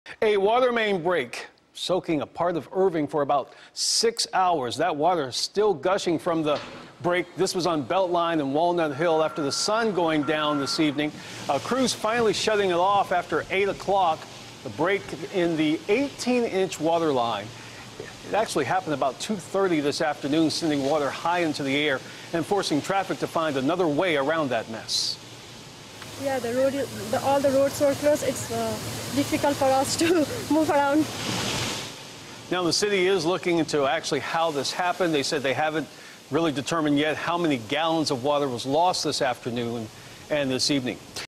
Breaking: Large Water Main Break In Irving
A large water main break in Irving was near W Walnut Hill Lane and N Belt Line Road Sunday afternoon.